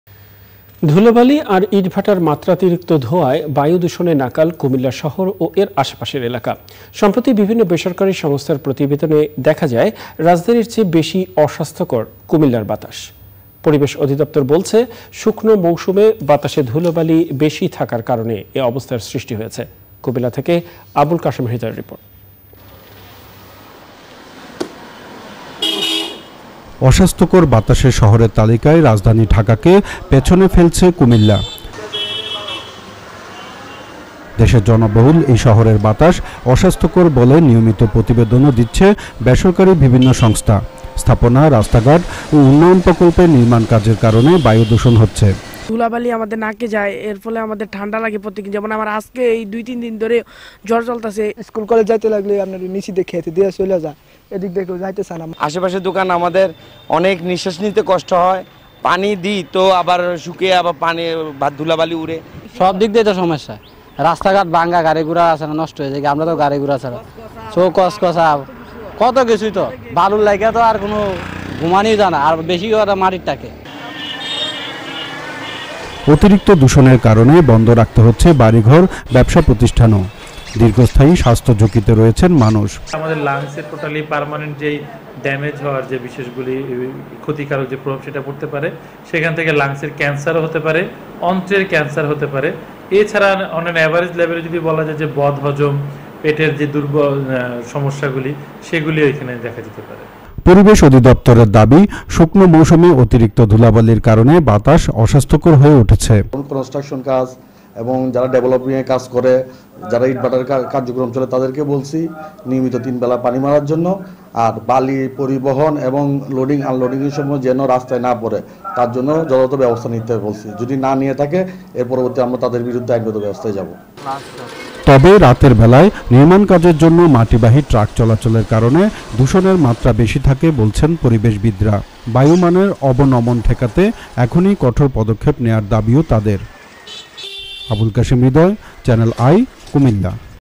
বায়ু দূষণে রাজধানীর চেয়েও বেশি অস্বাস্থ্যকর কুমিল্লা
ধূলোবালি আর ইটভাটার মাত্রাতিরিক্ত ধোঁয়ায় বায়ু দূষণে নাকাল কুমিল্লা শহর ও এর আশপাশের এলাকা। সম্প্রতি বিভিন্ন বেসরকারি সংস্থার প্রতিবেদনে দেখা যায়, রাজধানীর চেয়ে বেশি অস্বাস্থ্যকর কুমিল্লার বাতাস। পরিবেশ অধিদপ্তর বলছে, শুকনো মৌসুমে বাতাসে ধূলোবালি বেশি থাকার কারণে এ অবস্থার সৃষ্টি হয়েছে। কুমিল্লা থেকে আবুল কাশেম হৃদয়ের রিপোর্ট।

worldwide.